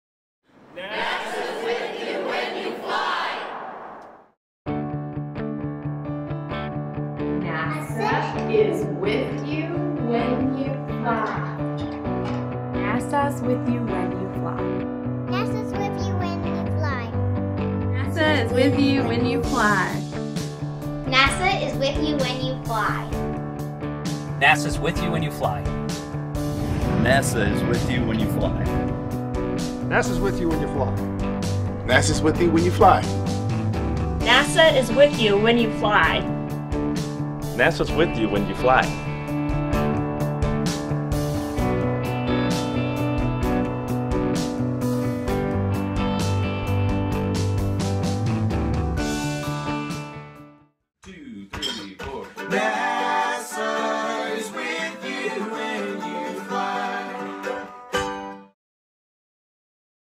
NASA is with You When You Fly
Did you know that NASA's with you when you fly? There's NASA-developed technology on board every U.S. aircraft and in every U.S. air traffic control tower.